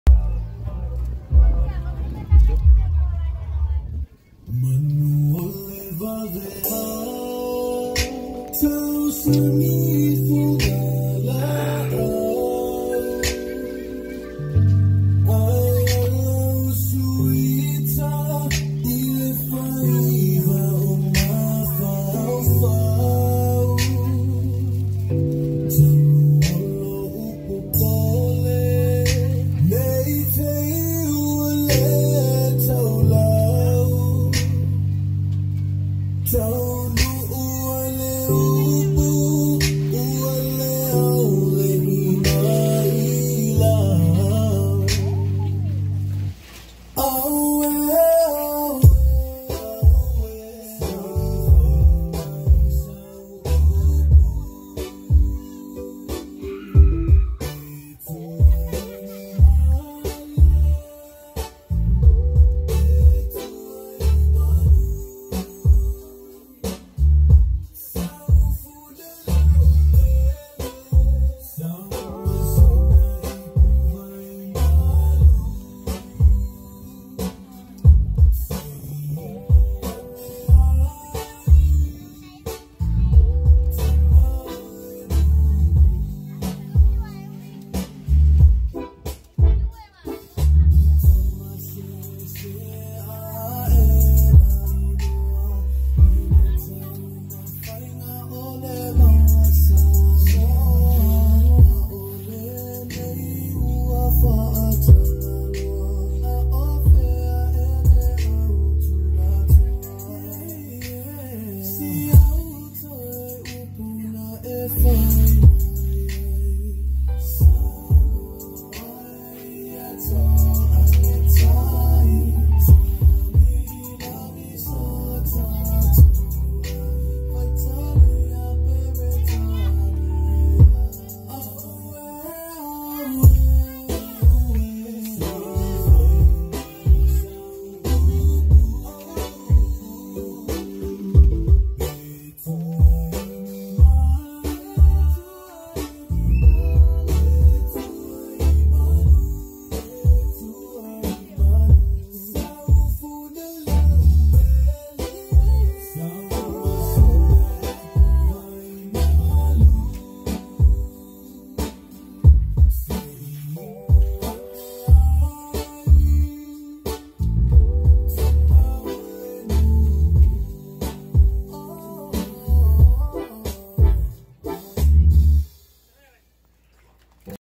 Happy Birthday Jisam Kaisha & Jimma Enja Enos! 💝
Happy 1st birthday to baby Jisam Kaisha and our very loved role model,Jimma Enja Enos! Mwe ñe kwjako ak eltan peim ej bedwot ippem kajojo! Love and Missu Dearly! Jerammon elap nan Jisam Kaisha im Jimma Enja Enos! 💕💓💞💝💘💖